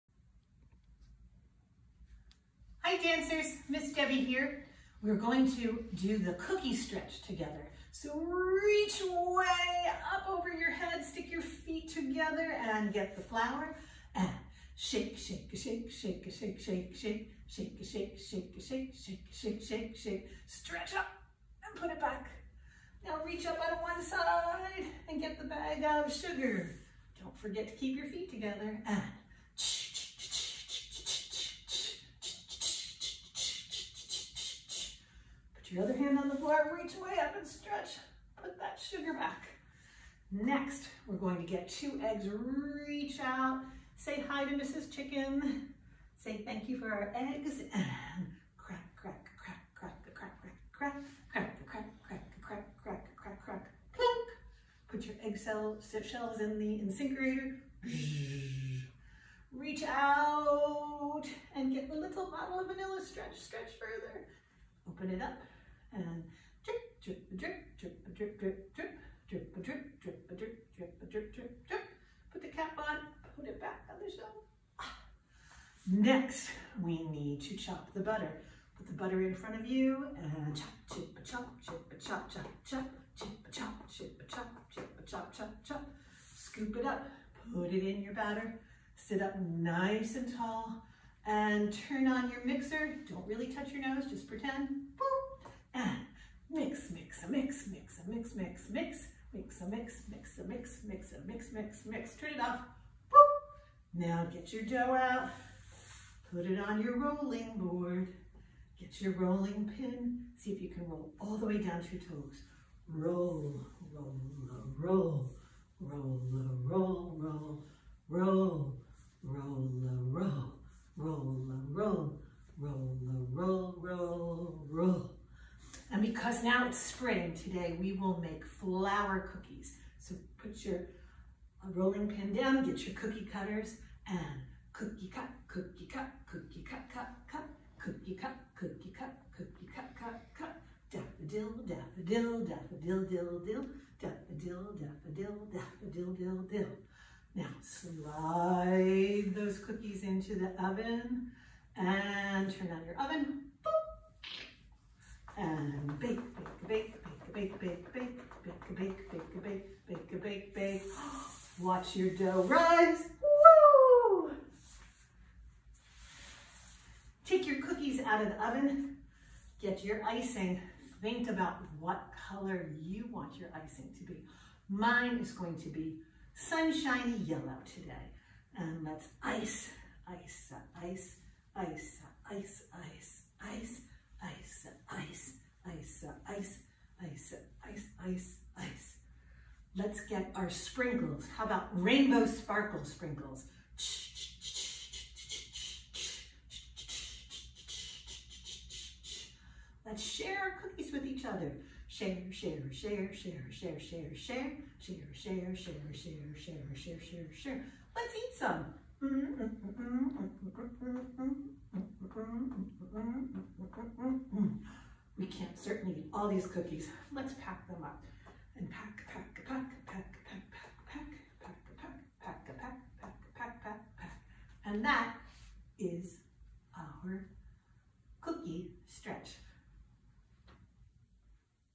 The Cookie Stretch
Enjoy stretching your arms and legs with the Cookie Stretch